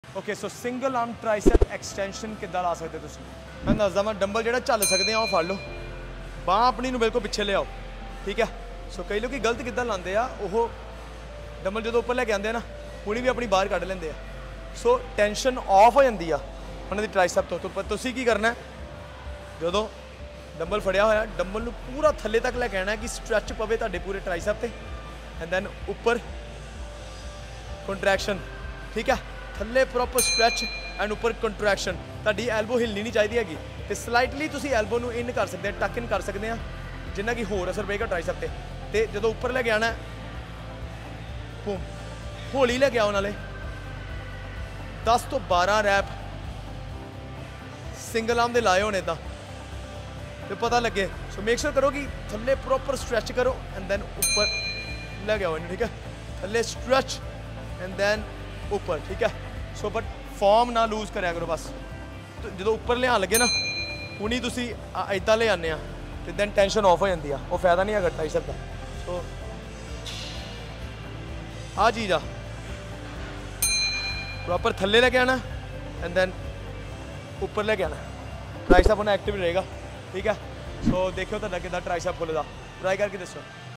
How To Perform Single-Arm Triceps Extension?
Elevate Your Triceps Game! Watch this quick tutorial on how to nail the Single-Arm Triceps Extension. Grab a dumbbell that challenges you and let's sculpt those triceps together!

Remember, form is key! Don't be one of those who miss out on the gains because of improper technique. Follow the steps in the video to ensure you're targeting those triceps effectively.

Perform 10-12 reps per arm for maximum impact. Consistency is key, so make this exercise a regular part of your routine for optimal results.

Need guidance or personalized training? I've got your back! Let's crush those fitness goals together!